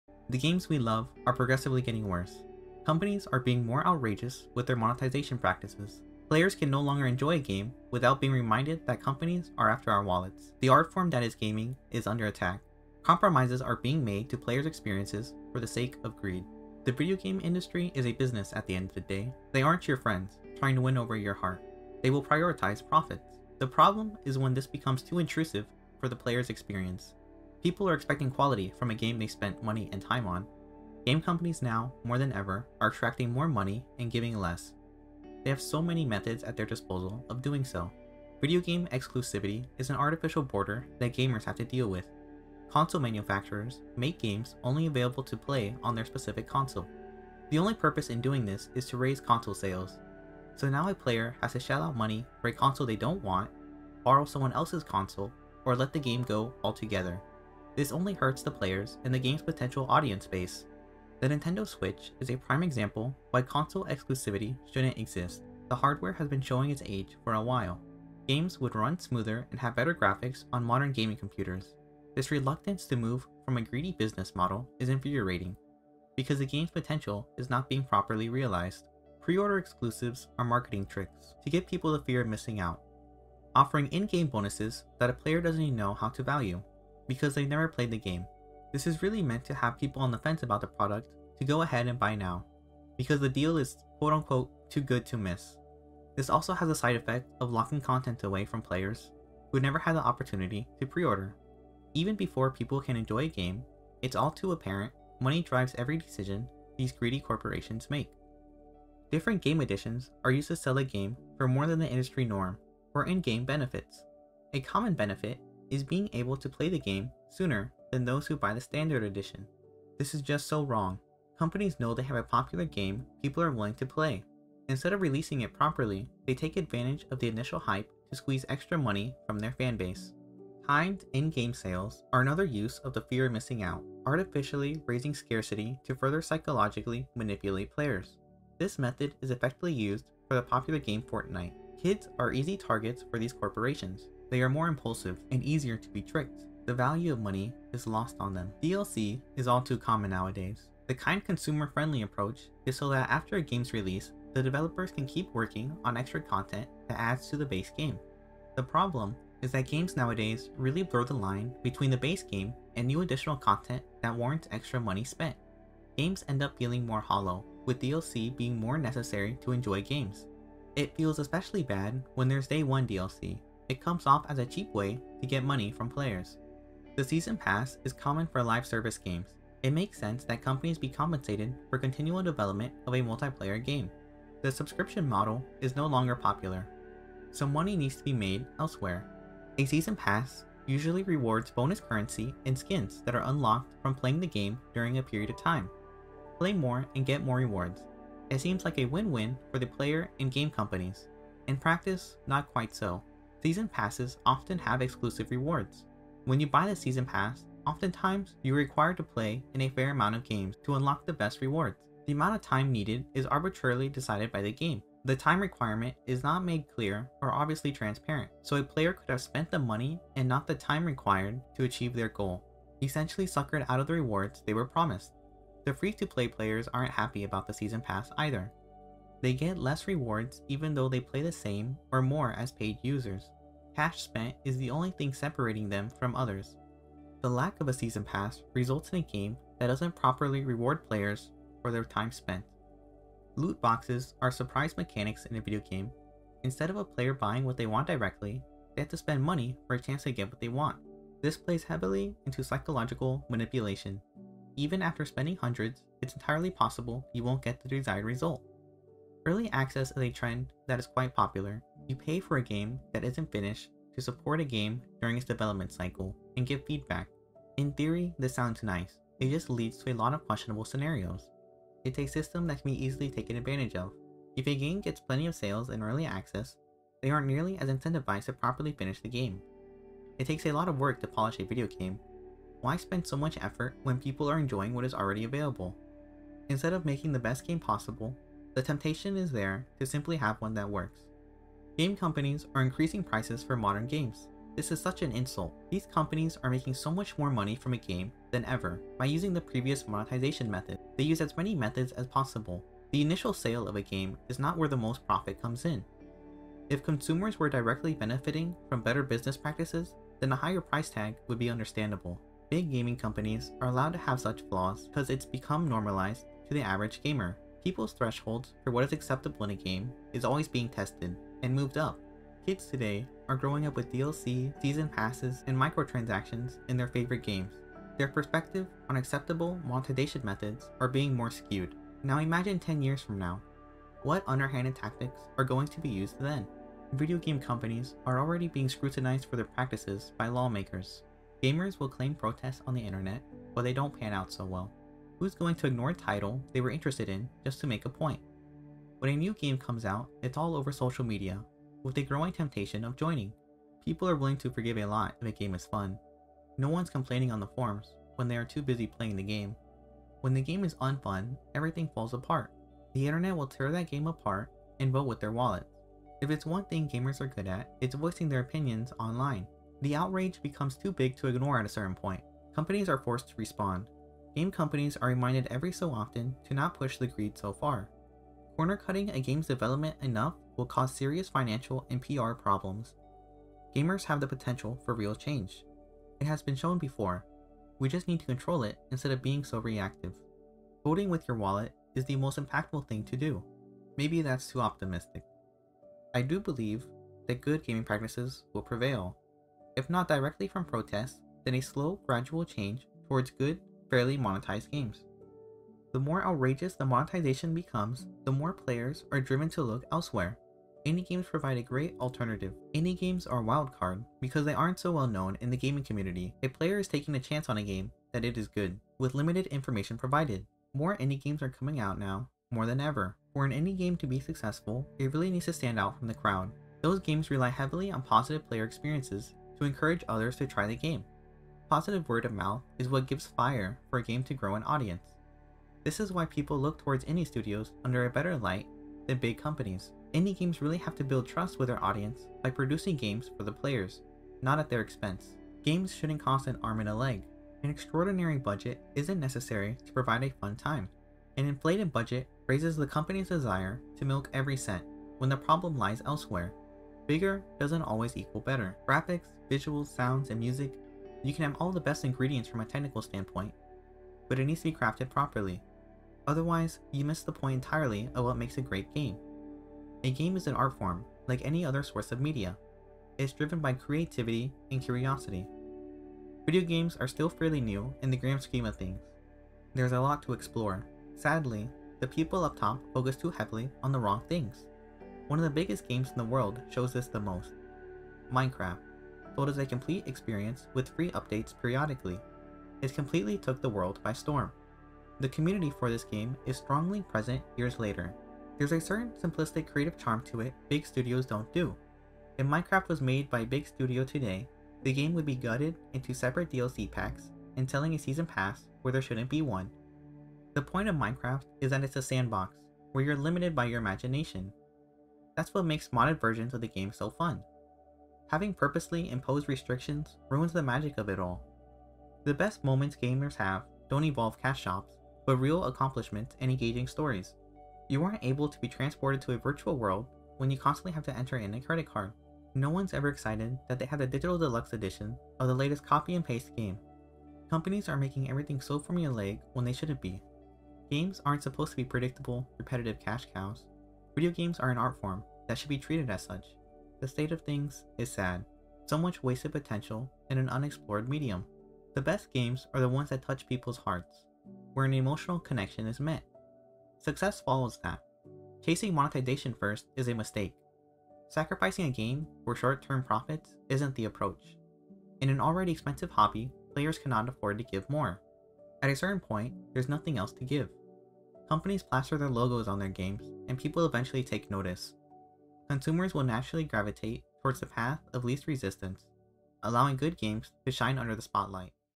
The Sad State of Gaming: Monetization | Video Essay
The video games we love are progressively getting worse. The artform is under attack. The common monetization methods in place are so damaging. Loot boxes, microtransactions, season passes, and so much more are damaging video games. Compromises are being made to player’s experiences for the sake of greed. Big companies are pushing the boundaries of what is acceptable. The players are the ones that ultimately pay the price. The current state of video games have to change. Monetization can only go so far.